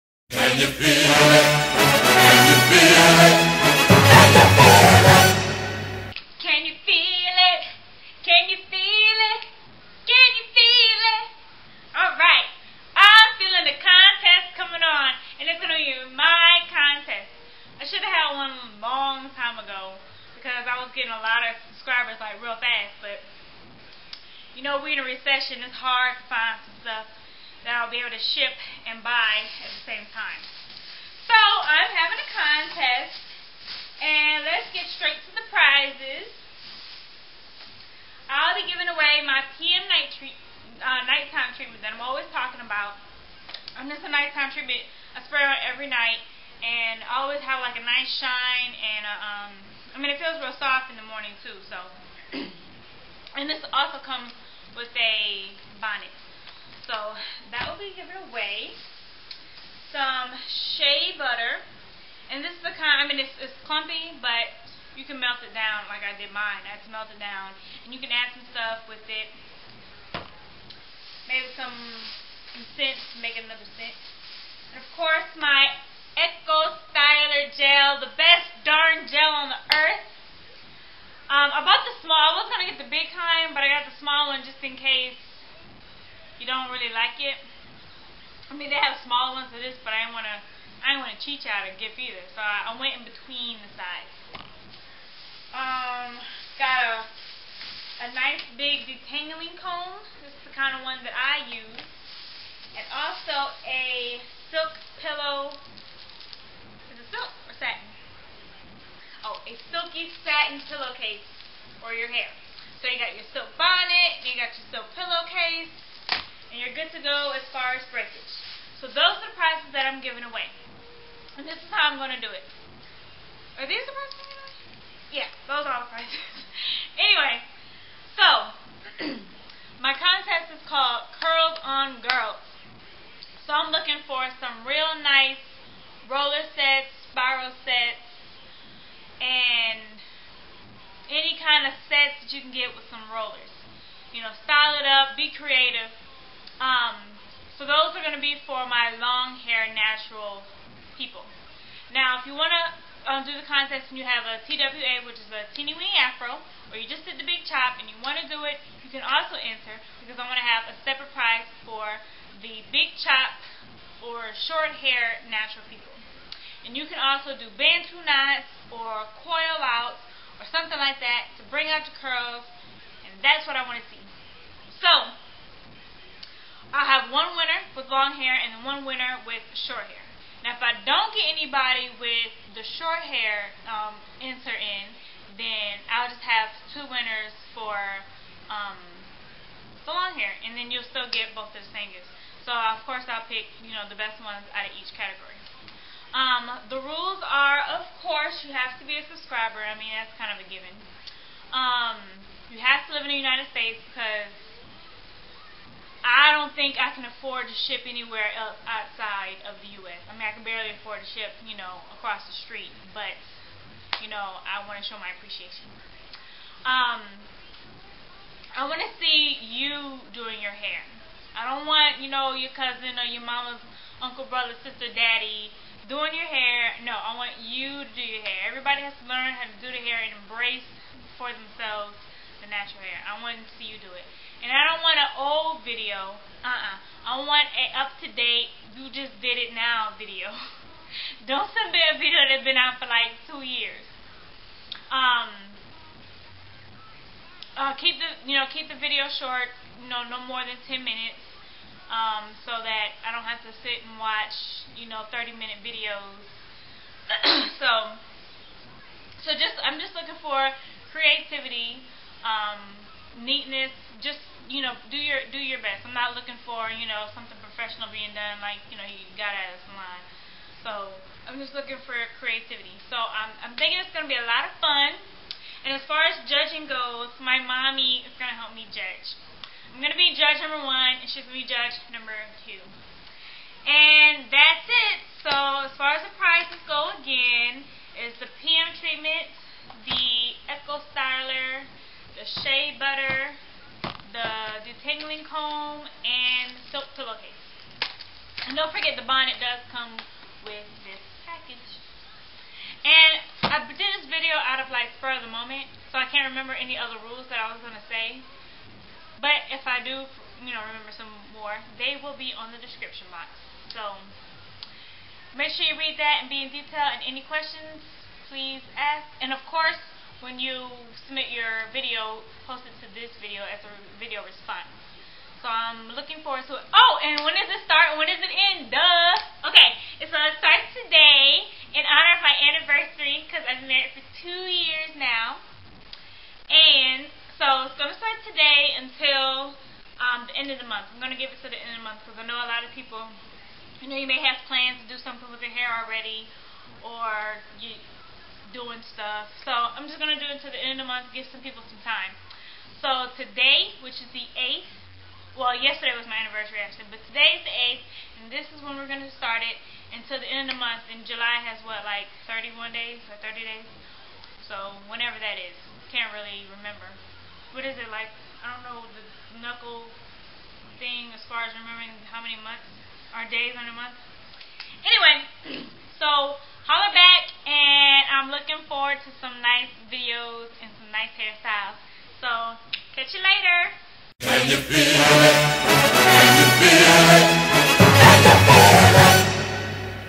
CONTEST: CURLS ON GURLS***CLOSED******
This will be my first contest but not my last. I want to see nice roller sets, sprial sets, bantu knots or coil outs. There will be one winner for the naturals with length and on winner for someone with shorter hair. The ones with shorter hair feel free to curl your hair with comb coils or bantu knots. The ones with longer hair curl your hair with rollers or rods or straws if you so wish, but don't feel you can't do bantu knots or comb coils too. Contest starts July8 2009 till July 31 2009. Now here are the rules.
RULES:
2. Must live in USA for shipping reasons(or Canada)
3. Must be natural or transitioning to go natural
4. Must be a new video
5. Must do your own hair. (no one else doing your hair for you)
6. Willing to give me your address so i can ship it. ( don't worry i won't harrass you) lol
7. Must be positive and ready for some fun.